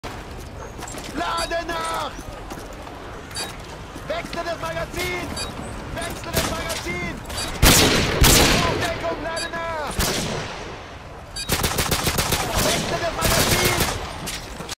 Drystreak won't end.
hey people! I just hit that sick non setup hitmark all across favela 10 mins ago and decided to upload it to stay active :) please leave a like & comment if you enjoyed!  watch in 720p60!